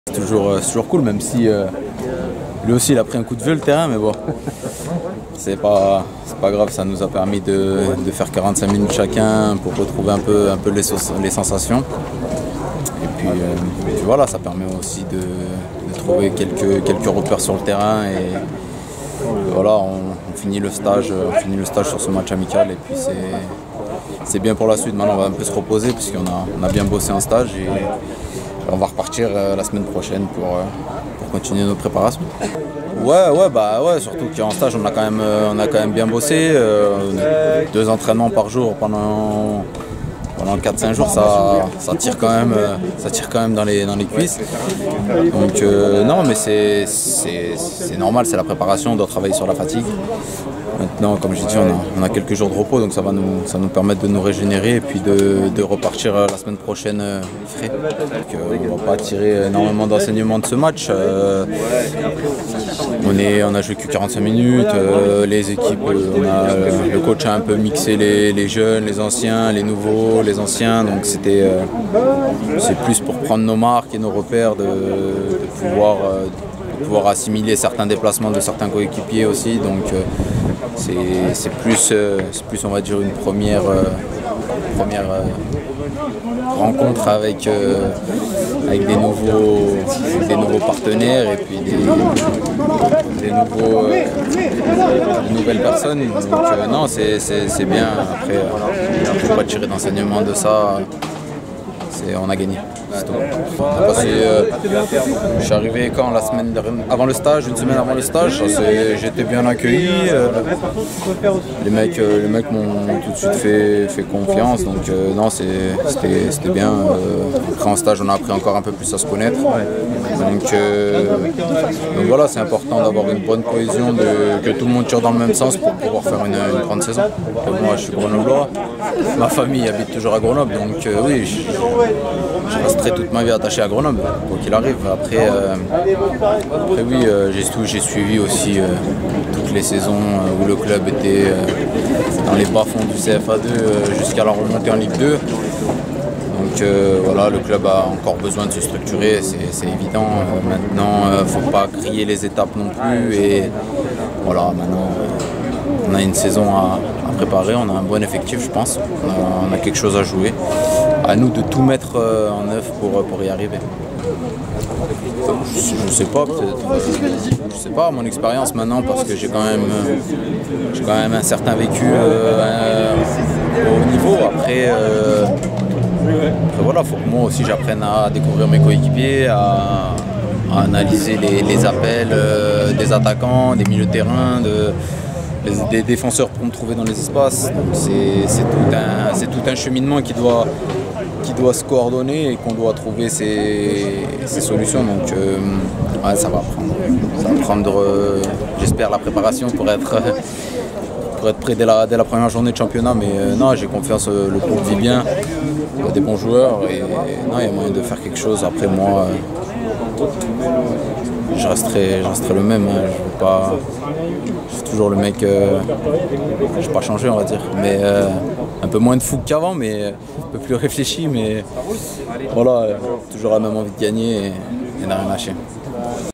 Yoric Ravet (GF38) : "J'ai toujours la même envie de gagner et de ne rien lâcher"
Le nouvel attaquant du Grenoble Foot 38 Yoric Ravet s'est exprimé à l"issue de la rencontre amicale remportée face au Sporting Club de Lyon ce vendredi 17 juillet 2020.

+ d'infos :